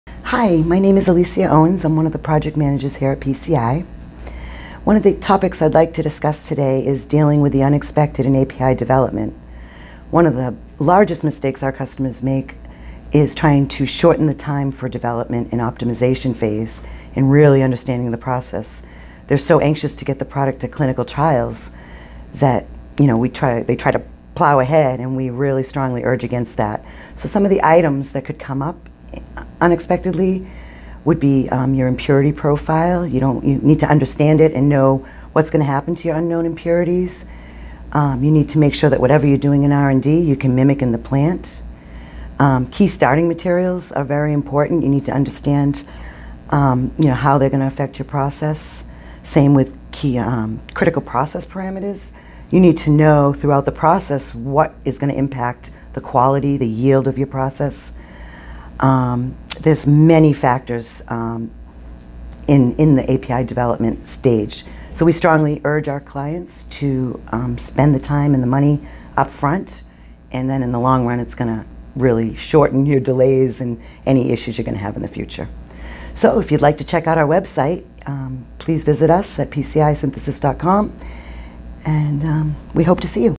Dealing with the Unexpected in API Development
Alesia Owens, Project Manager at PCI Synthesis talks about API Manufacturing from a project managers perspective.